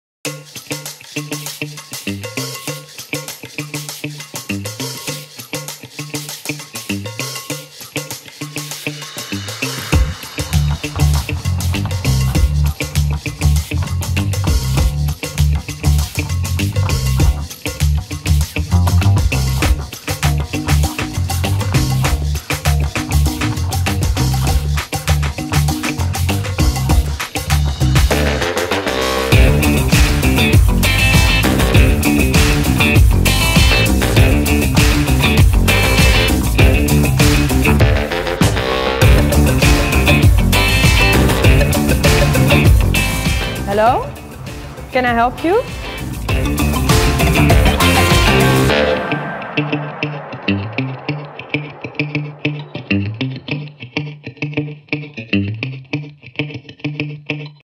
Hombli Smart Doorbell 2 - See always who’s at your door faster and smarter (Google version)
Meet the Hombli Smart Doorbell 2! The newest and improved version of our popular smart video doorbell. It features a 25% faster response time, option for wired installation and the battery lasts almost twice as long as its predecessor. You’re able to check even faster who’s standing in front of your door, home, or away.